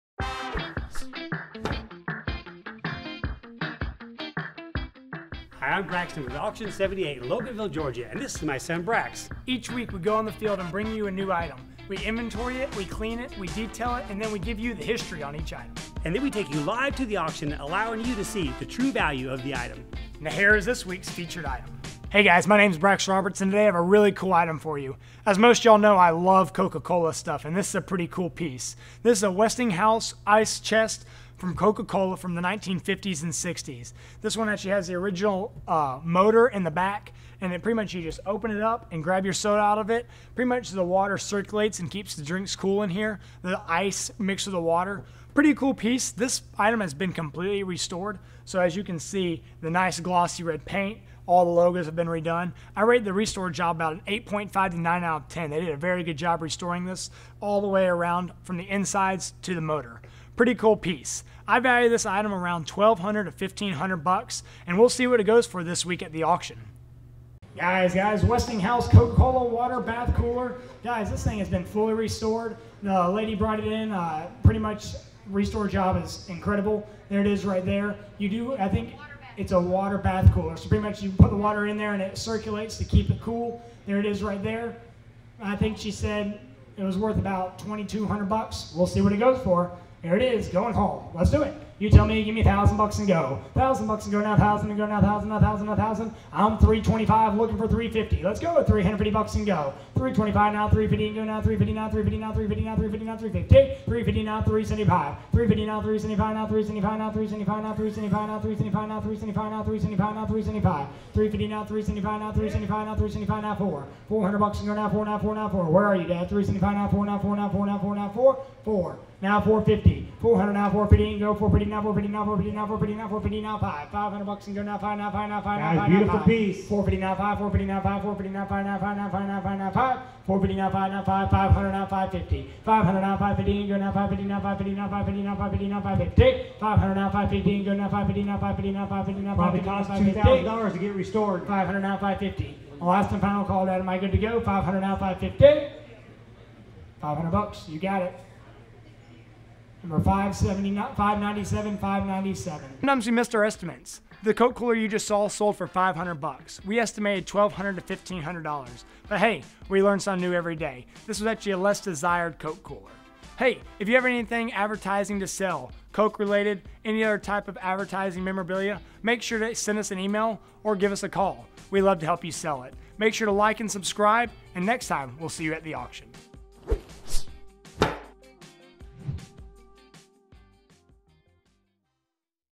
What's It Worth? - Vintage Westinghouse Coca Cola Ice Chest
Welcome back to another weekly Auction 78 "What's it Worth?". This week we show you just how much Westinghouse Coca Cola refurbished from the 1950's and 60's with original motor in the back. This item has been completely restored from the inside out! Check out this video to see how much we guess it will sell for, compared to how much it actually sold for!!